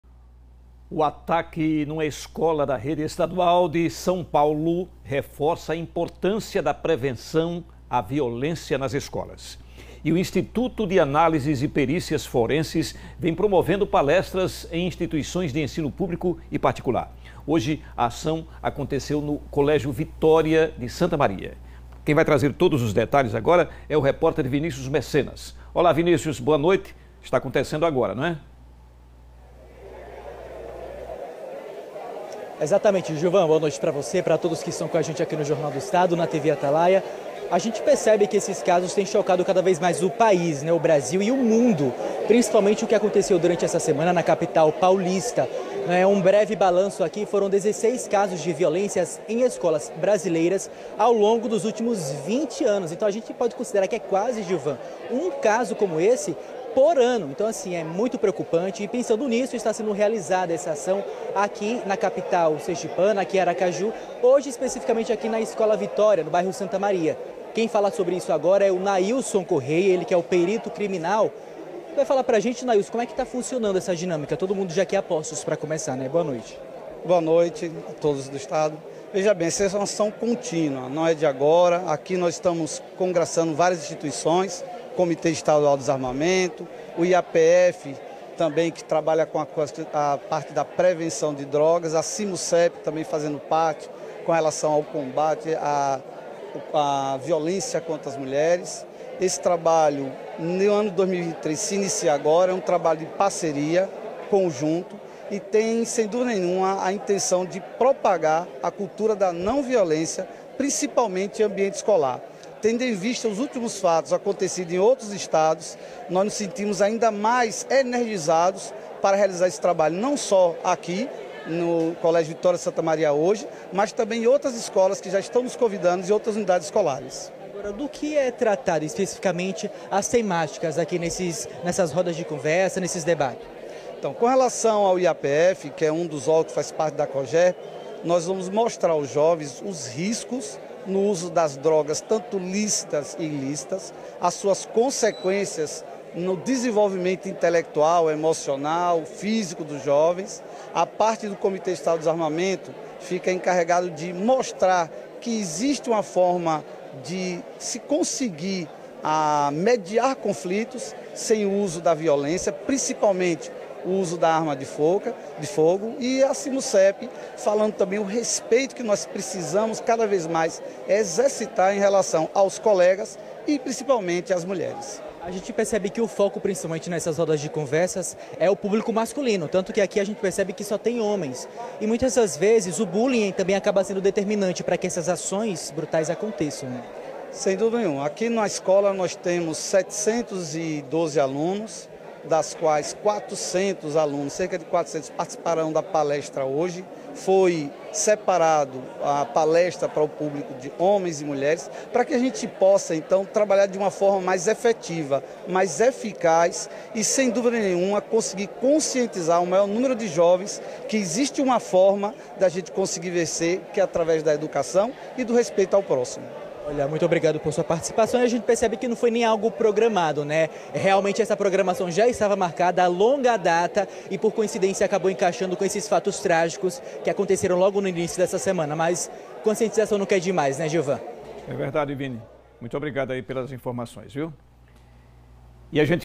Ataque em escola de SP reforça a importância da prevenção à violência nas escolas - Jornal do Estado
O instituto de análises e perícias forenses vem promovendo palestras em instituições de ensino público e particular.